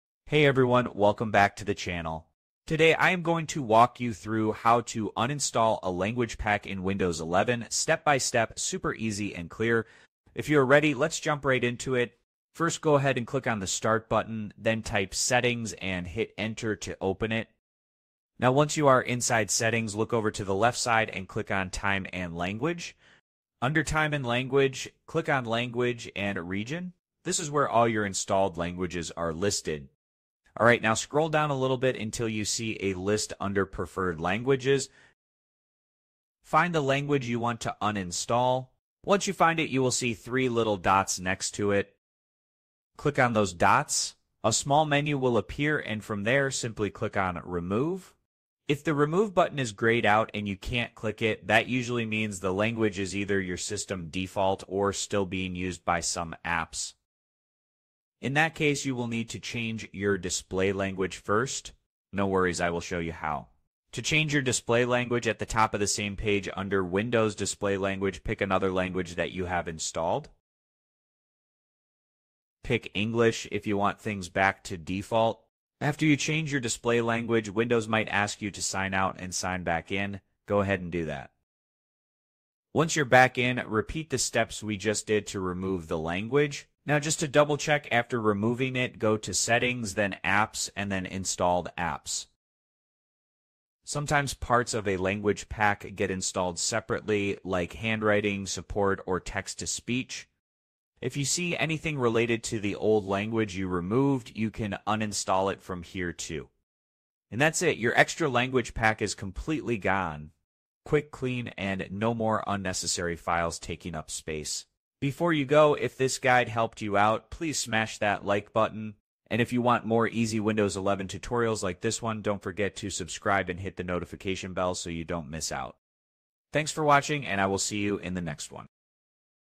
Remove/Uninstall Language Packs in Windows 11 — Quick & Easy Step-by-Step Guide
Tired of extra language packs cluttering your Windows 11 system? Want to free up space and simplify your interface? In this clear and practical tutorial, we’ll show you exactly how to uninstall language packs in Windows 11 — even if one is set as your system default. No confusion. No unnecessary steps. Just clean, simple results.

🔹 What You’ll Learn in This Video:
✅ How to access and navigate the Language & Region settings
✅ How to remove any language pack from your system
✅ What to do if the Remove button is grayed out
✅ How to change the Windows display language
✅ How to check for leftover components like handwriting or speech support

⏱️ Timestamps:
00:00 – Intro & Why You Might Want to Remove a Language Pack
00:12 – Navigating to Language Settings
00:30 – Removing a Language Pack
00:48 – What to Do If "Remove" Is Grayed Out
01:22 – Final Cleanup & Removing Leftover Language Components

Removing language packs you no longer use can:
Improve system performance
Free up valuable disk space
Reduce background processes
Clean up the user interface

This guide is especially helpful for shared PCs, student laptops, or any device that came with extra languages pre-installed.

🔁 Share it with others who might need it